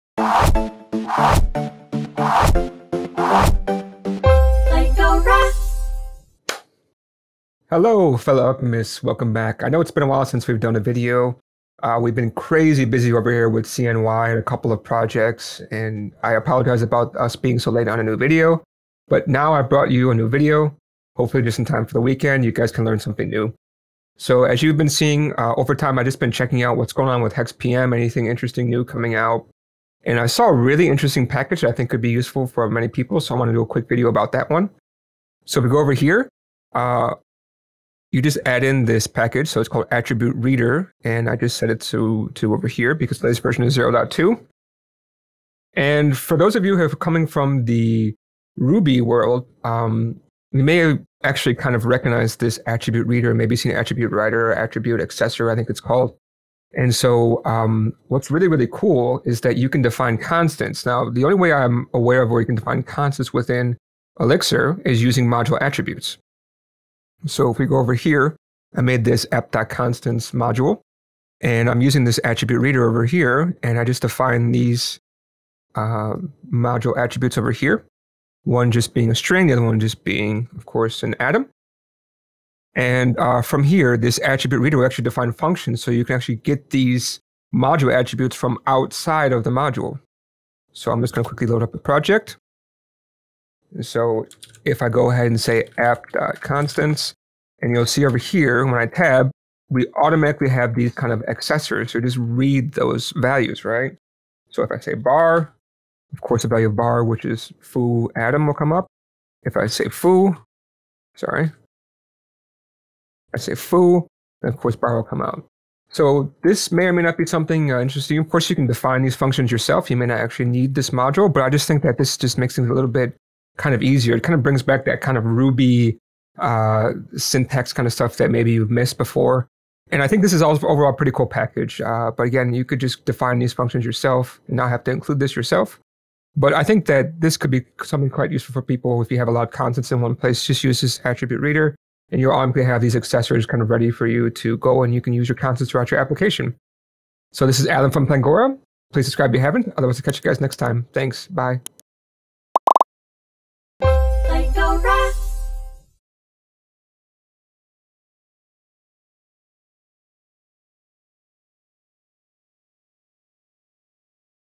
AttrReader
Hello, fellow alchemists! We are back with a new episode to show you more about AttrReader in Phoenix. Don't miss out!